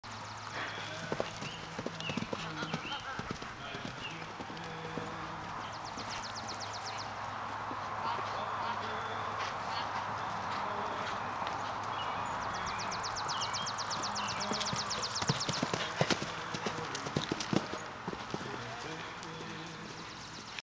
"Varia" jumping 2'6''-3 foot
Varia is a 2001, 16.1 hand Hanoverian mare.